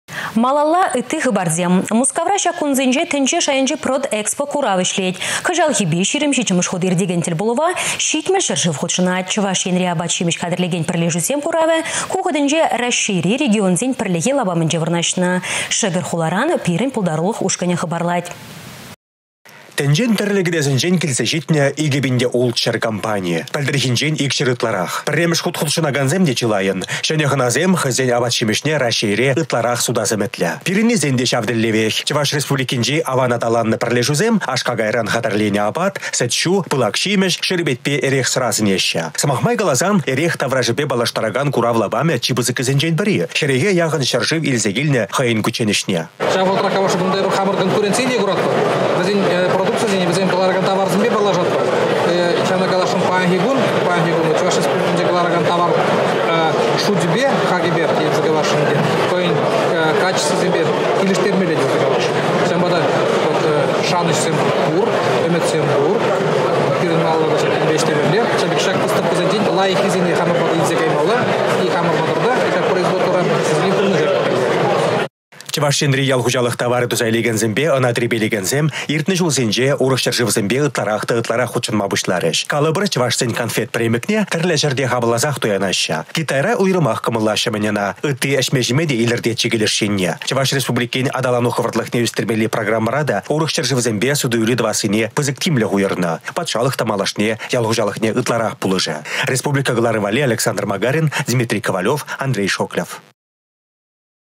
Тĕнчери апат-çимĕç куравĕнче
Малалла - ытти хыпарсем. Мускавра çак кунсенче тěнче шайěнчи «Продэкспо» курав ĕçлет. Кăçалхипе 27-мĕш хут иртекен тĕлпулăва 70 çĕршыв хутшăнать. Чăваш Енри апат-çимĕç хатĕрлекен пĕрлешӳсем куравĕ ку хутĕнче Раççейри регионсен пĕрлехи лапамĕнче вырнаçнă. Шĕкĕр хуларан пирĕн пултарулăх ушкăнĕ хыпарлать.